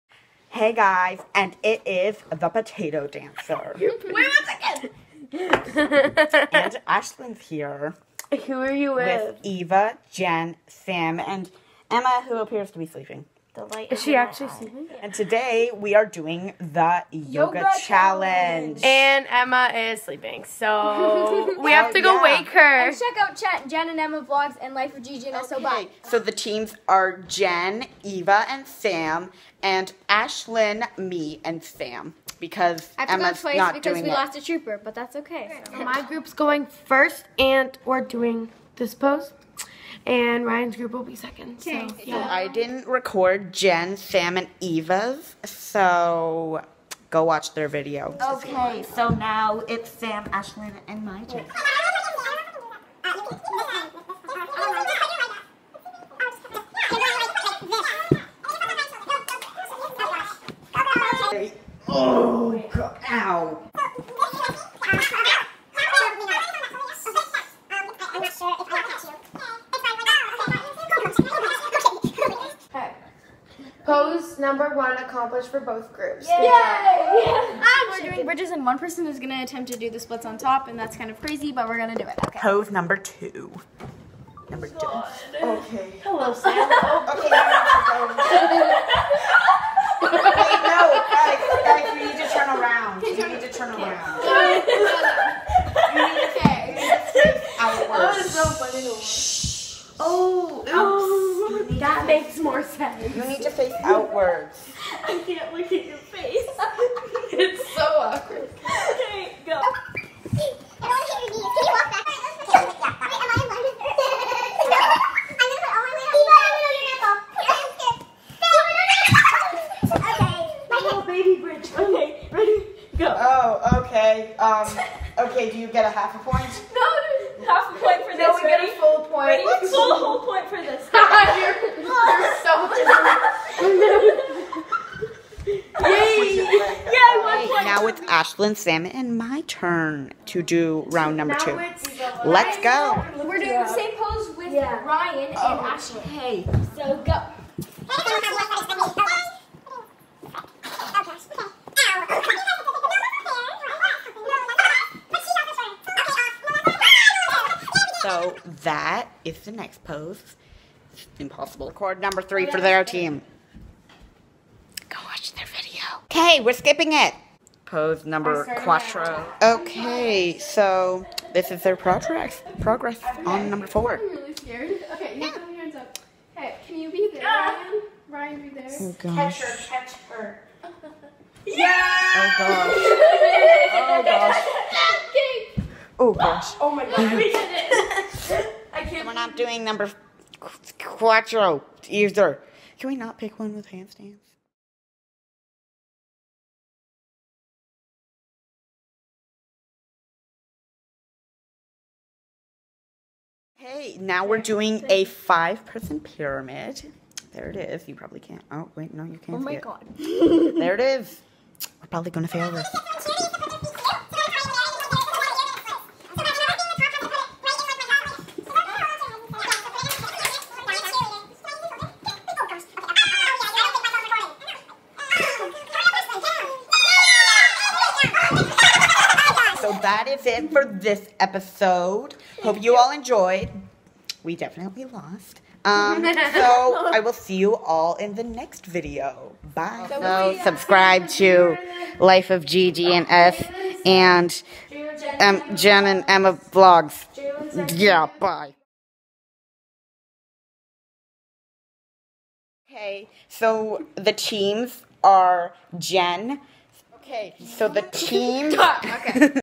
The Yoga Challenge Ft.\Jen and Emma Vlogs/,Life_Of_GG&S,Eva and Ashlyn
This is my first challenge on my channel so I hope you like it. I hope you like the video!

Jen and Emma’s channel

Sam’s channel